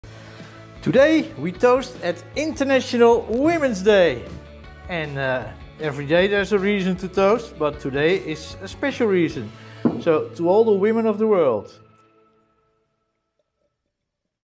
International Women Day in Cairanne
Today we toast at International Women's Day 2021 from the Cru Côtes du Rhône village Cairanne in the French Wine region Provence. To all the Women of the world we say cheers with a Red Wine.